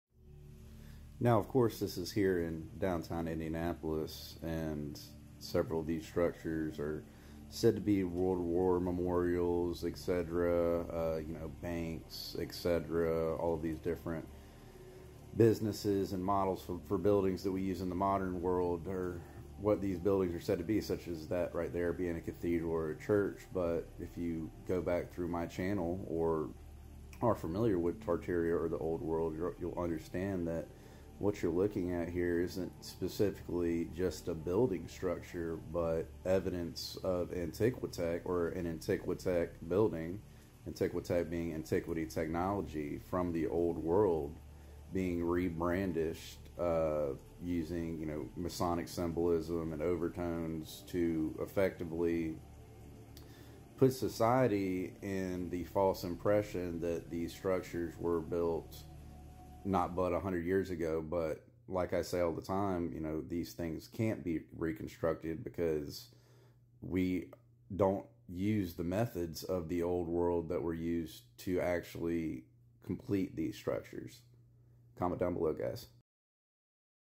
Old World Indianapolis
A short video exploring the downtown area of Indianapolis, Indiana.